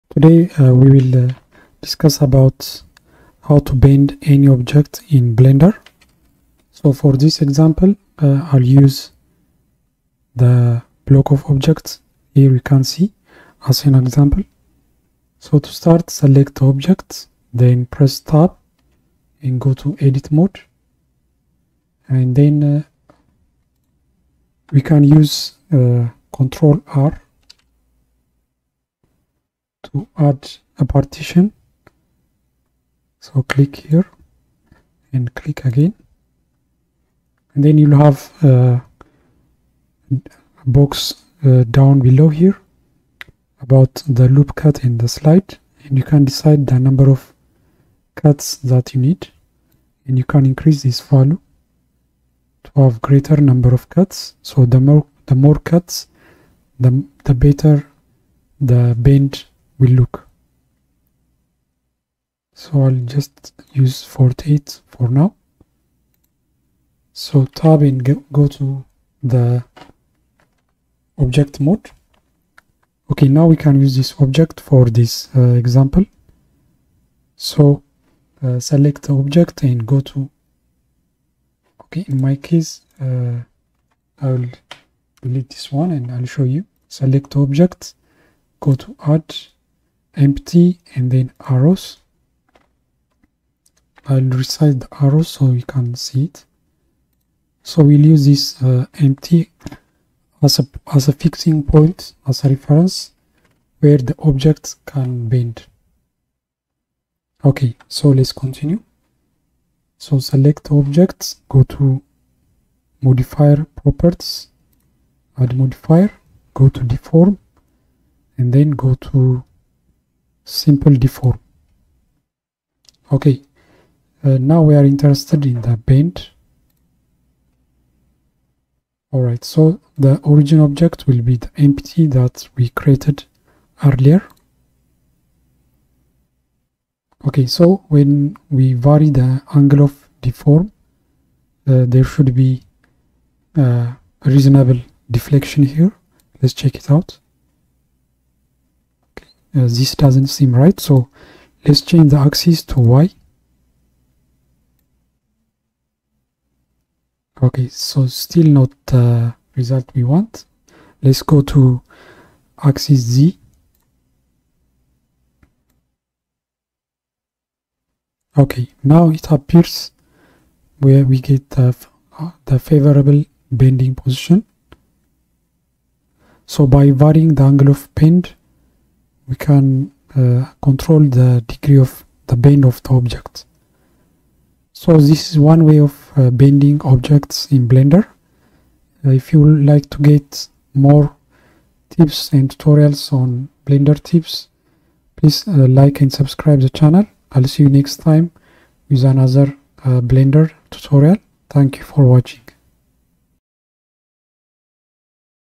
How to bend any object in Blender
In this tutorial i'll explain about a technique that can be used to bend any object in Blender.